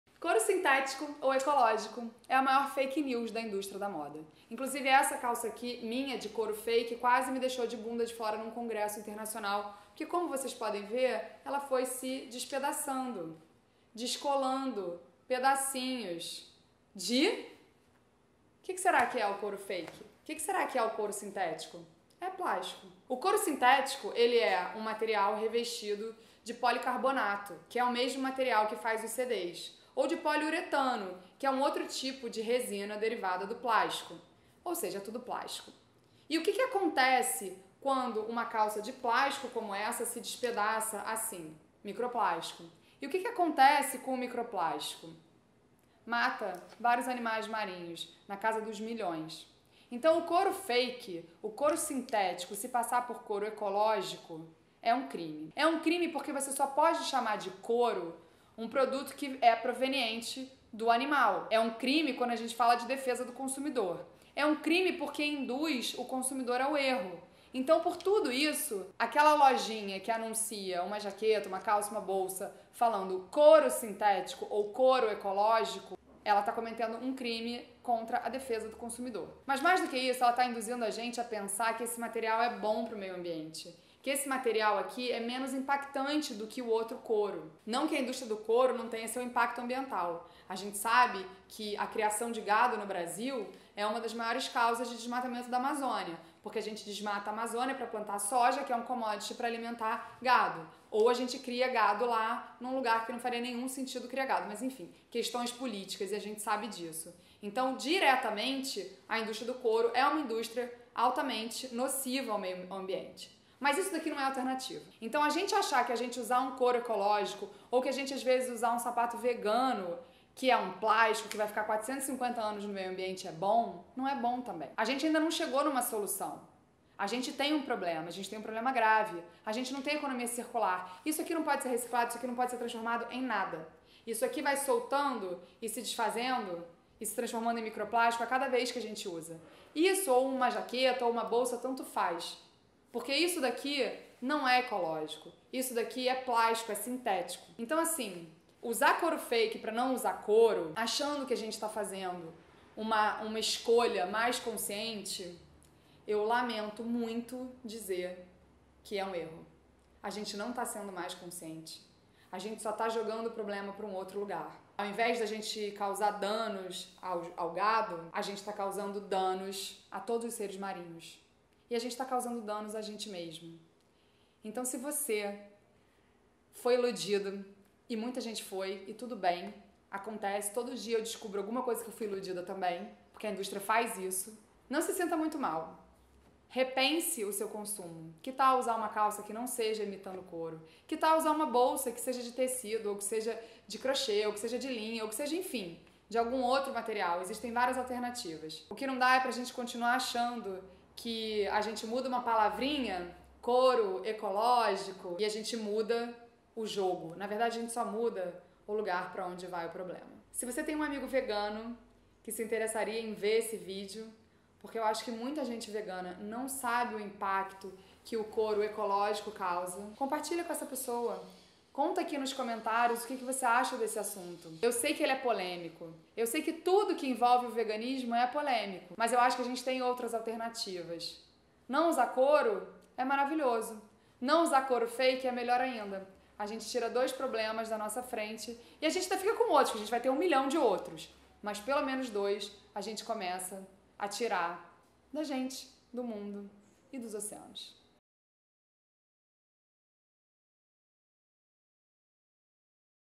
Couro ecológico é fake news!
Não caia na fake news do "couro ecológico" que de eco, não tem nada. Esse couro é feito de plástico e se desgasta muito fácil, deixando um rastro de microplásticos pelo caminho. A indústria do couro animal é péssima pro meio ambiente, mas não é com o couro fake que você vai estar salvando o planeta. Dá um play e entenda!

Para saber mais sobre nosso maior agente de transformação, o copo